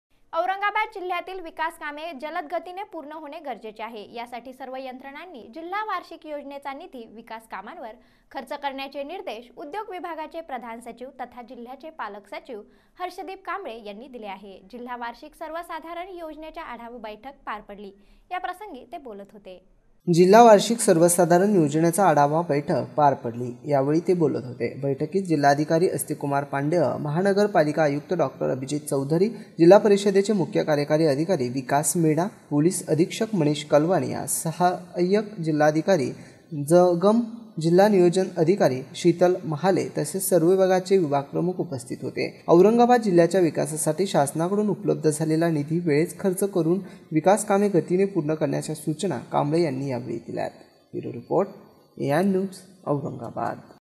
औरंगाबाद : सर्व विभागांनी समन्वयातून जिल्ह्याचा सर्वागीण विकास साधावा.
सर्व विभागांनी समन्वयातून जिल्ह्याचा सर्वागीण विकास साधावा
                                  -      पालक सचिव हर्षदीप कांबळे
      औरंगाबाद दि. 28 (जिमाका) – जिल्ह्यातील विकास कामे गतीने पूर्ण होणे गरजेचे आहे. यासाठी सर्व यंत्रणांनी परस्पर समनव्य ठेवण्याचे निर्देश देत उद्योग विभागाचे प्रधान  जिल्हा वार्षिक योजनेचा निधी सचिव विकास कामांवर खर्च करण्याचे निर्देश उद्योग विभागाचे प्रधान सचिव तथा जिल्ह्याचे पालक सचिव हर्षदीप कांबळे यांनी दिले.

            जिल्हा वार्षिक सर्वसाधरण योजनेच्या आढावा बैठक पार पडली त्यावेळी ते बोलत होते. बैठकीस जिल्हाधिकारी आस्तिककुमार पाण्डेय, महानगरपालिका आयुक्त डॉ.अभिजीत चौधरी, जिल्हा परिषदेचे मुख्य कार्यकारी अधिकारी विकास मीना, पोलीस अधीक्षक मनीष कलवानिया, सहायक जिल्हाधिकारी श्री.जंगम, जिल्हा नियोजन अधिकारी शितल महाले तसेच सर्व विभागाचे विभाग प्रमुख उपस्थित होते.

            औरंगाबाद जिल्ह्याच्या विकासासाठी शासनाकडून उपलब्ध झालेला निधी वेळेत खर्च करून विकासकामे गतीने पूर्ण करण्याच्या सूचना श्री. कांबळे यांनी यावेळी दिल्या.